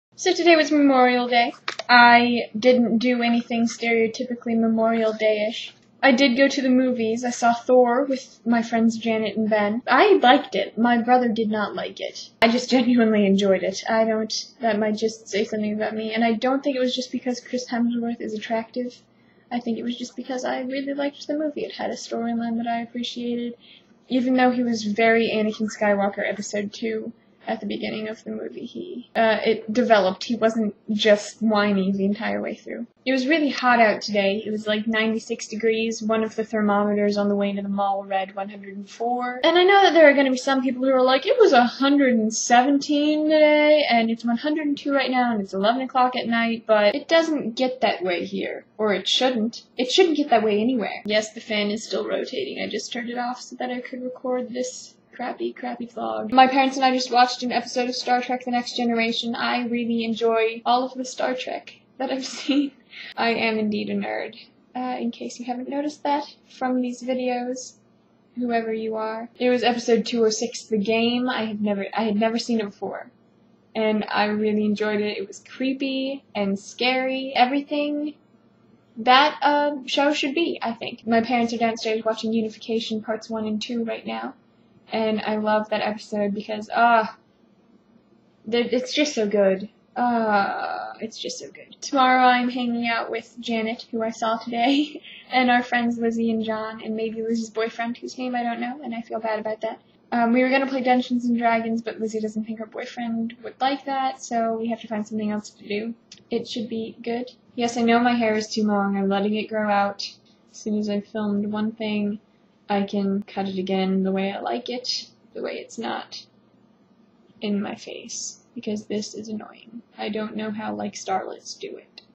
Day 15: The Norse God of Thunder and Also My Hair
Dear FM,

I recently got a Tumblr, which Google Chrome does not believe is actually a word unitl you add it to the dictionary, and I have one follower but am following 17 people. One of them posts pictures of cats on my dash and they are so cute. You know how we feel about cats.

Today was the day that you went to see Thor but got stuck in the third row because it took Janet heinously long to park. Why were there so many people at the mall? Shouldn't they have been cooking out or something? It was like Mothers' Day 2009 all over again, when we thought that we and Bryan and Mom and Dad were going to be the only ones seeing Star Trek XI, but then there were all of these other awesome nerds there WITH THEIR MOMS!!!

Love,
NM